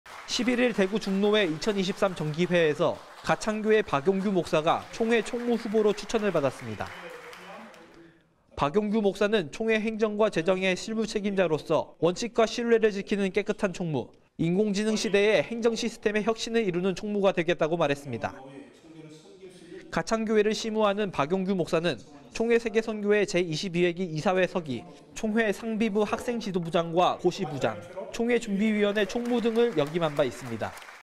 예장(합동) 대구중노회 박용규 목사 총회총무 후보추천 (대구, 배진우) l CTS뉴스
11일 대구중노회 2023정기회에서 가창교회 박용규 목사가 총회 총무 후보로 추천을 받았습니다.

박용규 목사는 총회 행정과 재정의 실무책임자로서 원칙과 신뢰를 지키는 깨끗한 총무, 인공지능 시대에 행정 시스템의 혁신을 이루는 총무가 되겠다고 말했습니다.

가창교회를 시무하는 박용규 목사는 총회세계선교회 제22회기 이사회 서기, 총회 상비부 학생지도부장 고시부장, 총회준비위원회 총무 등을 역임한 바 있습니다.